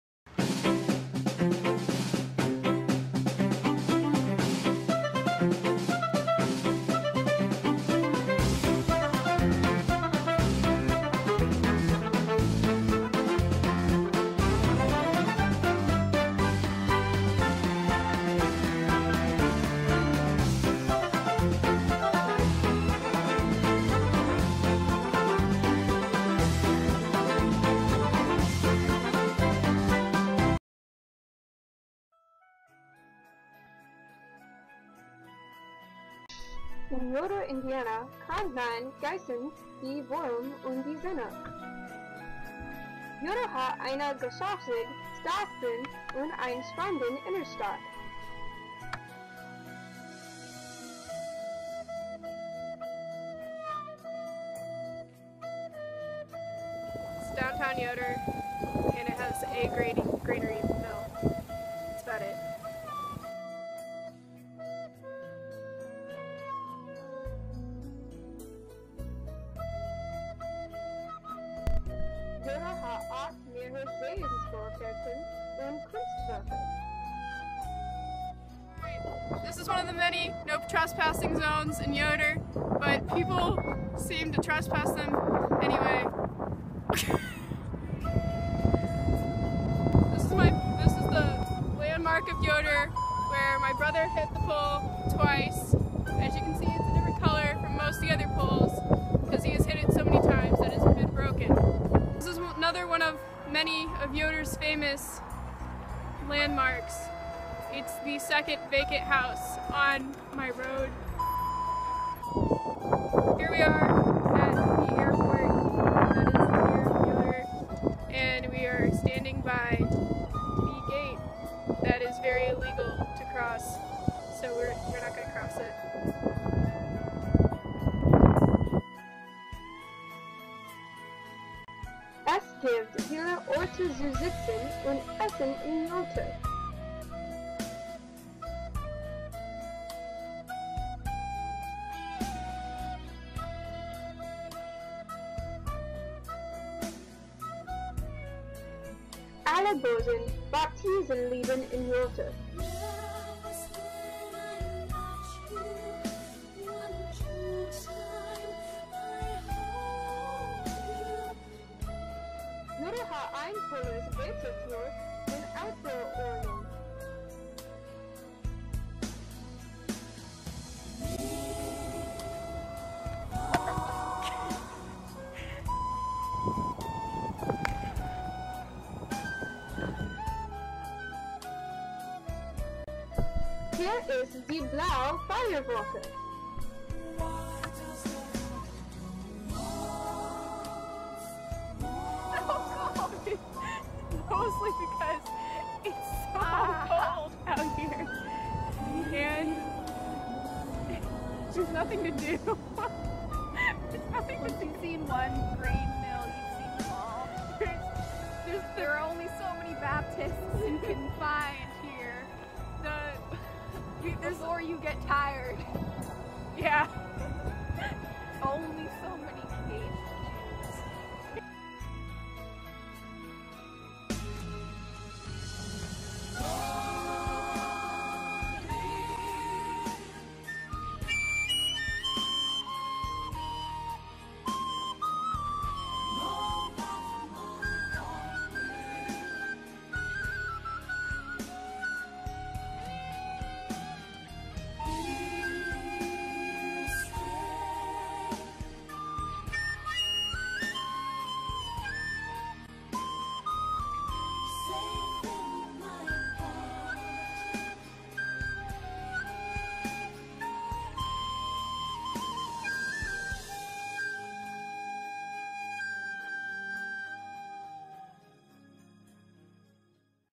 Yoder, IN | My high school German project | One of the greatest cinematic master pieces of all time
All the German is wrong, but wow you can't beat those scenic Yoder, IN views.